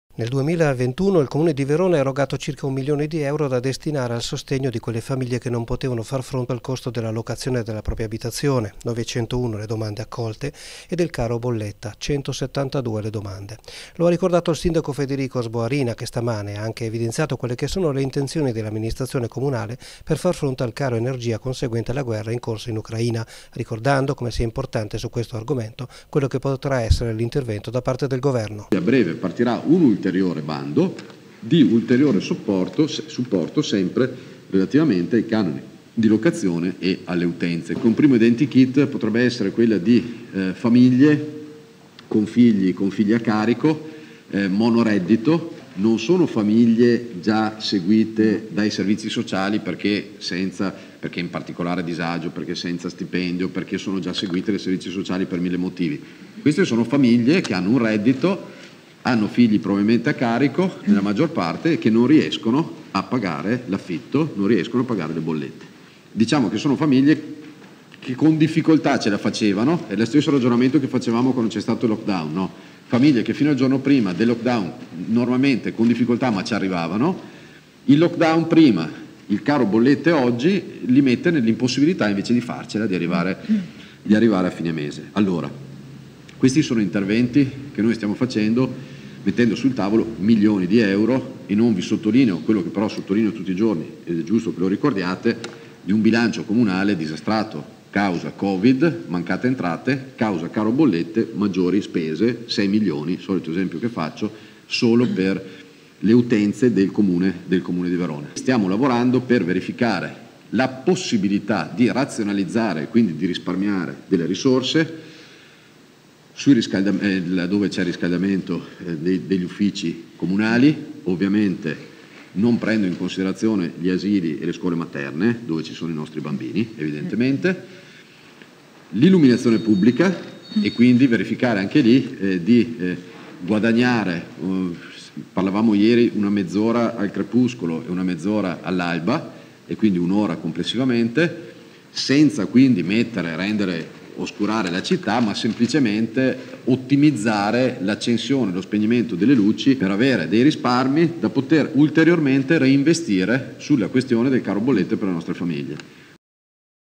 Caro bollette, gli aiuti erogati e quelli previsti dal comune di Verona
901 domande di sostegno al pagamento dell’affitto per le abitazioni e 172 sostegni per le bollette è quanto è stato erogato nello scorso anno dal Comune di Verona. Lo ha illustrato questa mattina il sindaco Sboarina preannunciando gli interventi per l’anno in corso.

DIchIarazione di Federico Sboarina - Sindaco di Verona